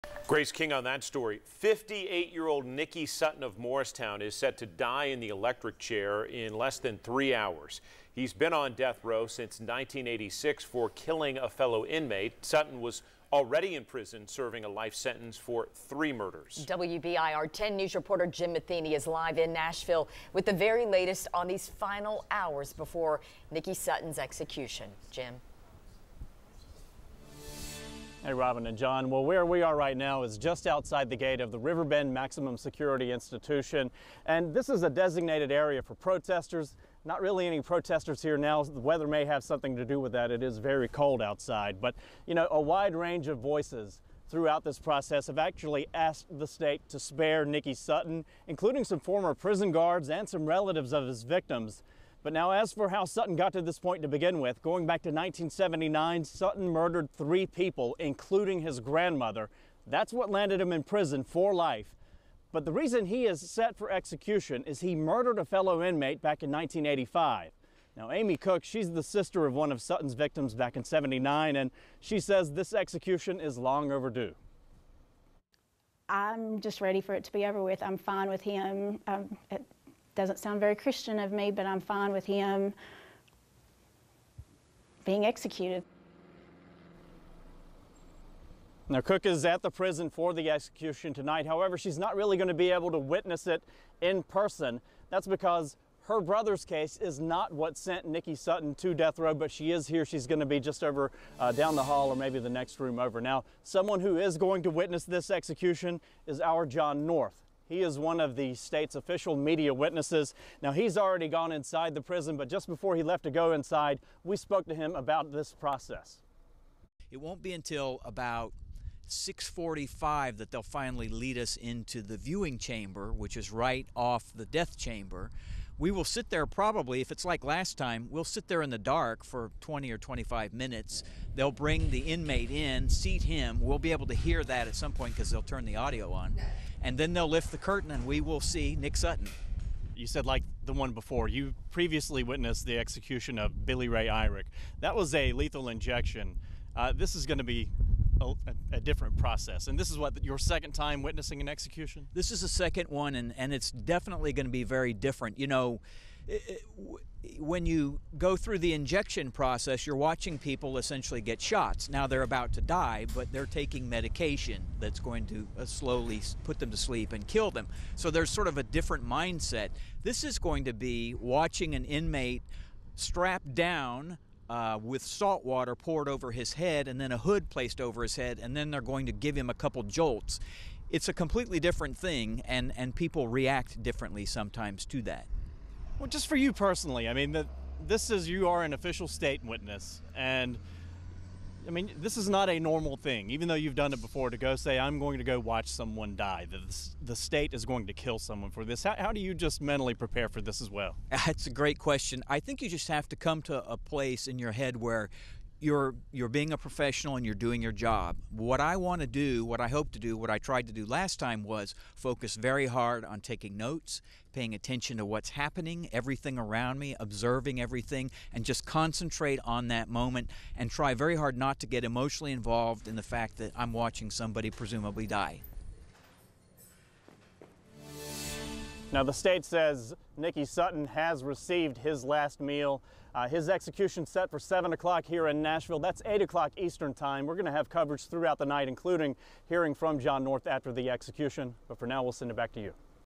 Sutton to die by electric chair Thursday night
Nicky Sutton, 58, is set to die in the electric chair after being on death row since 1986 for killing a fellow inmate.